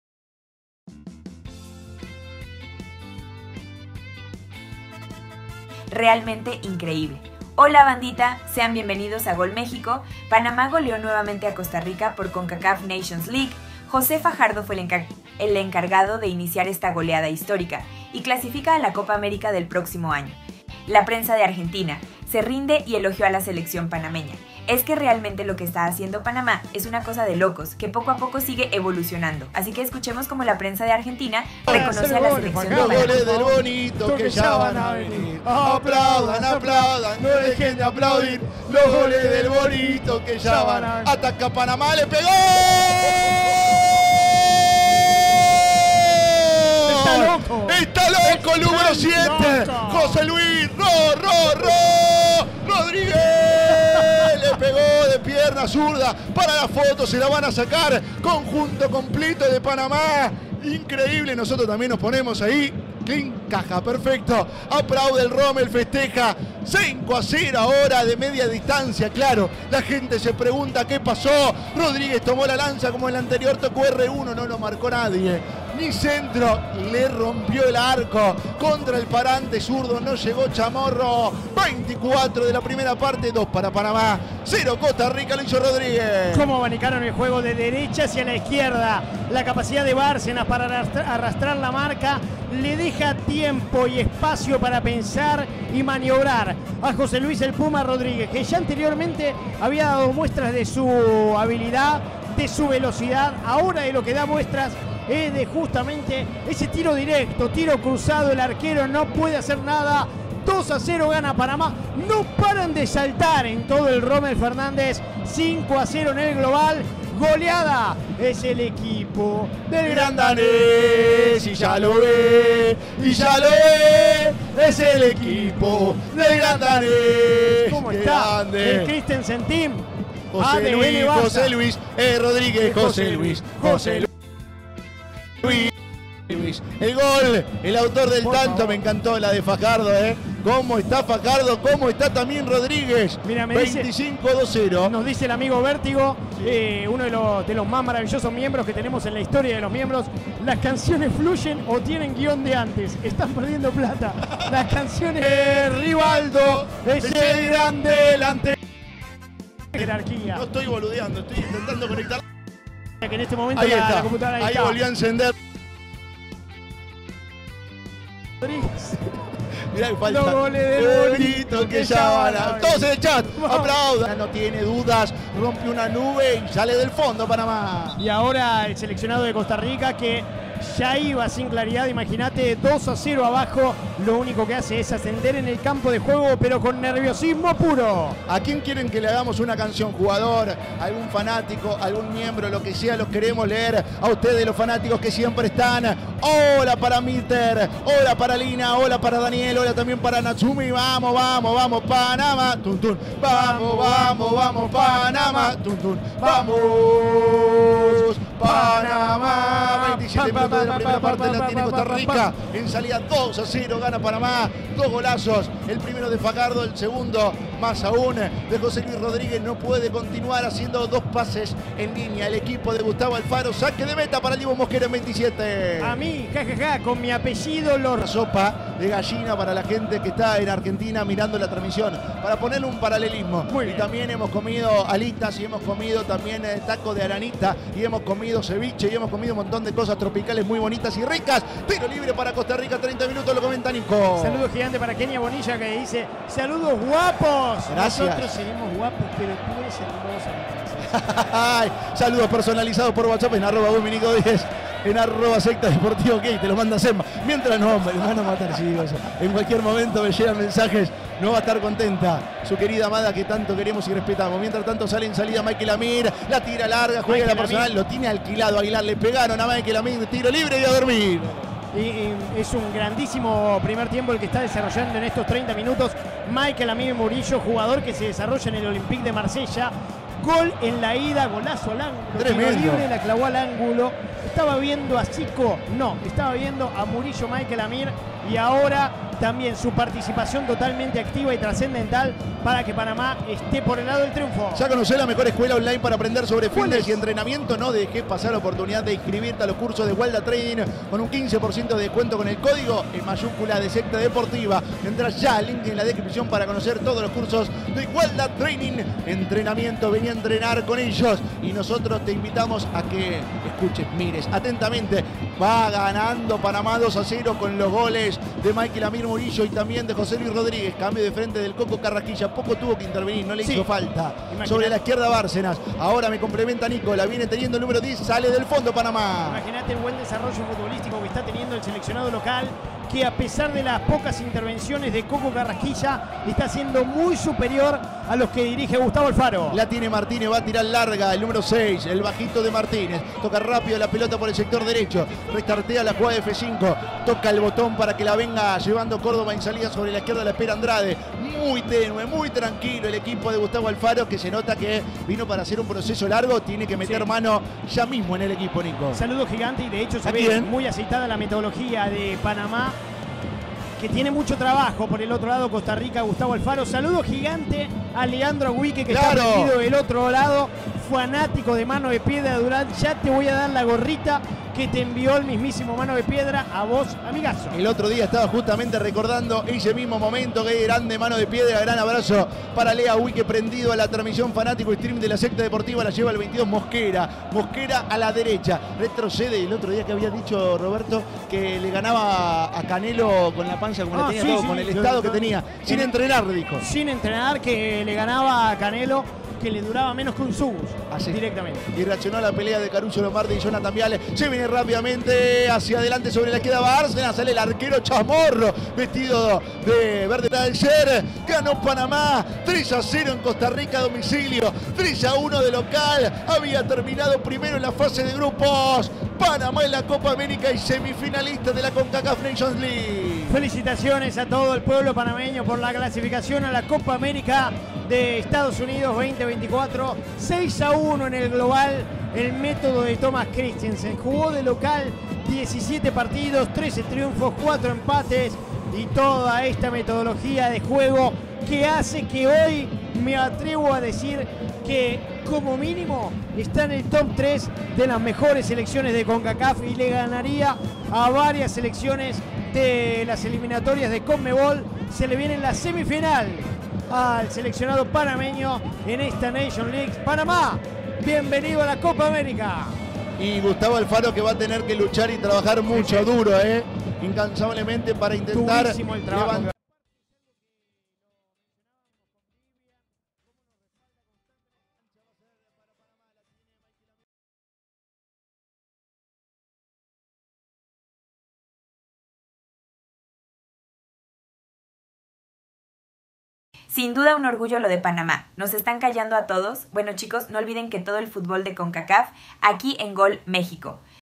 PRENSA DE ARGENTINA FELICITA A PANAMA !! PANAMA BIEVENIDO A LA COPA AMERICA !! ERES UNA MAQUINA
PRENSA DE ARGENTINA FELICITA A PANAMA !! PANAMA BIEVENIDO A LA COPA AMERICA !! ERES UNA MAQUINA  ¡Avanza los ‘Canaleros’! Panamá derrotó 3-1 a Costa Rica, por Liga de la Naciones Concacaf

Panamá va ganando el encuentro ante costa rica.

Con goles de Fajardo, Puma y Barcenas, Panamá derrotó 3-1 a Costa Rica, por la Liga de la Naciones Concacaf. JOSE FAJARDOOOOO🔥🇵🇦⚽️